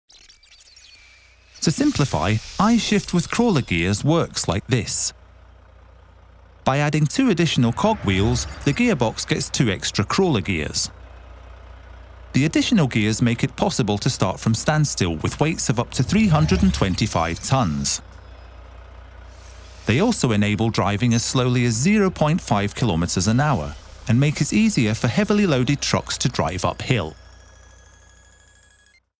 Slik funker I-Shift med krabbegir
LANSERING! I-Shift med krabbegir. Se animasjonen.
Ved å legge til to ekstra tannhjul får den nye versjonen av I-Shift to ekstra krabbegir. Her ser du hvordan krabbegiret fungerer.